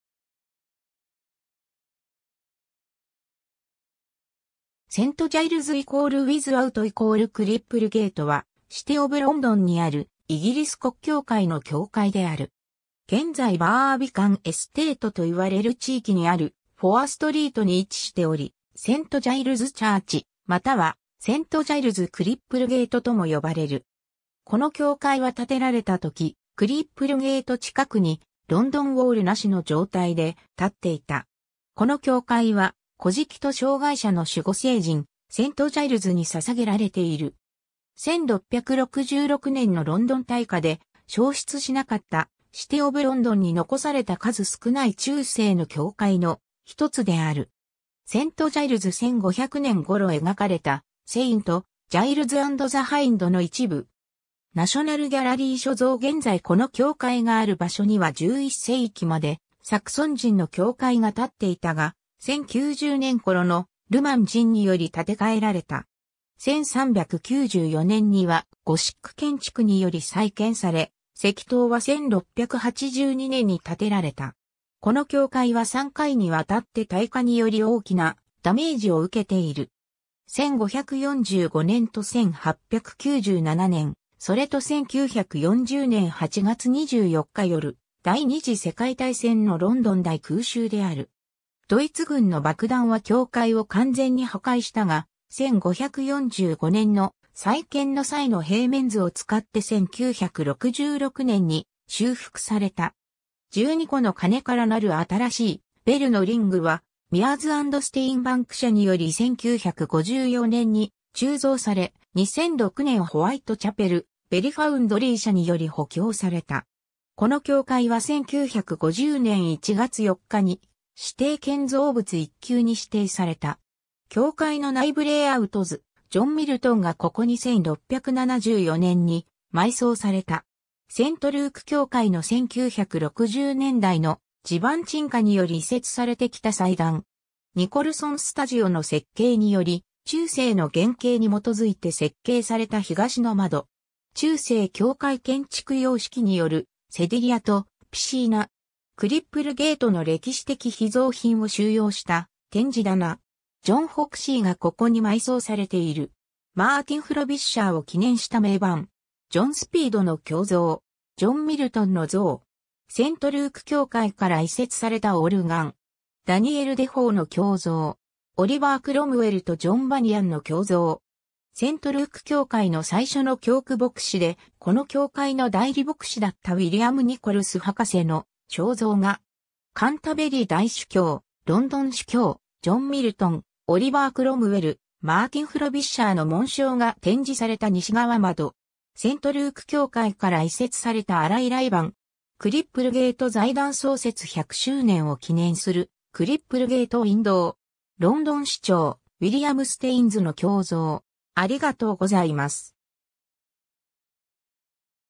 セント・ジャイルズ＝ウィズアウト＝クリップルゲート
シティ・オブ・ロンドンの建築物
セント・ジャイルズ＝ウィズアウト＝クリップルゲート (英: St Giles-without-Cripplegate) は、シティ・オブ・ロンドンにあるイギリス国教会の教会である。
現在バービカン・エステート (英語版) と言われる地域にあるフォア・ストリート (英語版) に位置しており、セント・ジャイルズ・チャーチ (St Giles' Church) 、またはセント・ジャイルズ・クリップルゲート (St Giles' Cripplegate) とも呼ばれる。
 この教会は建てられたとき、クリップルゲート (英語版) 近くに、ロンドン・ウォールなしの状態で、（つまり壁の外側に）立っていた。
この教会は、乞食と障がい者の守護聖人、セント・ジャイルズに捧げられている。
1666年のロンドン大火で焼失しなかった、シティ・オブ・ロンドンに残された数少ない中世の教会の一つである 。
 セント・ジャイルズ 1500年頃描かれた"Saint Giles and the Hind"の一部。
ナショナル・ギャラリー所蔵 現在この教会がある場所には11世紀までサクソン人の教会が立っていたが、1090年頃ノルマン人により建て替えられた。
1394年にはゴシック建築により再建され 、石塔は1682年に建てられた。
 この教会は3回にわたって大火により大きなダメージを受けている。
1545年と1897年 、それと1940年8月24日夜、第二次世界大戦のロンドン大空襲である。
ドイツ軍の爆弾は教会を完全に破壊したが、1545年の再建の際の平面図を使って1966年に修復された 。
 12個の鐘からなる新しい「ベルのリング」(Ring of bells) (英語版) はミアーズ・アンド・ステインバンク社により1954年に鋳造され、2006年ホワイトチャペル・ベル・ファウンドリー社 (Whitechapel Bell Foundry) (英語版) により補強された。
 この教会は1950年1月4日に、指定建造物 1級 (Grade I) に指定された。
 教会の内部 レイアウト図 ジョン・ミルトンがここに1674年に埋葬された。
 セント・ルーク教会 (St Luke's) (英語版) の1960年代の地盤沈下により移設されてきた祭壇。
 ニコルソン・スタジオ (Nicholson Studios) の設計により、中世の原型に基づいて設計された東の窓。
 中世教会建築様式によるセディリア (Sedilia) (英語版) と、ピシーナ (Piscina) (英語版)。
 クリップルゲートの歴史的秘蔵品を収容した展示棚。
 ジョン・フォクシーがここに埋葬されている。
 マーティン・フロビッシャーを記念した銘板。
 ジョン・スピードの胸像。
 ジョン・ミルトンの像 。
 セント・ルーク教会から移設されたオルガン 。
 ダニエル・デフォーの胸像。
 オリバー・クロムウェルとジョン・バニヤンの胸像。
 セント・ルーク教会の最初の教区牧師 (rector) で、この教会の代理牧師 (vicar) だったウィリアム・ニコルス博士 (Dr. William Nicholls) (英語版) の肖像画。
 カンタベリー大主教、ロンドン主教 (英語版) 、ジョン・ミルトン、オリバー・クロムウェル、マーティン・フロビッシャーの紋章が展示された西側窓。
 セント・ルーク教会から移設された洗礼盤 (font) 。
 クリップルゲート財団 (The Cripplegate Foundation) 創設100周年を記念するクリップルゲート・ウィンドウ (The Cripplegate Window) 。
 ロンドン市長ウィリアム・ステインズ (William Staines) (英語版) の胸像。